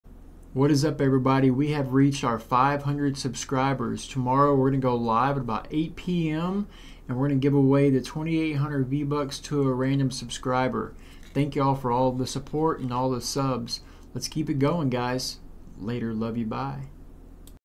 We have Reached 500 Subscribers!!!!
We have reached 500 Subs Everyone Thank you all for the love and Support and all the positivity. We will be picking a random Sub to receive 2,800 V-Buck tomorrow @ 8Pm Again thank all of you.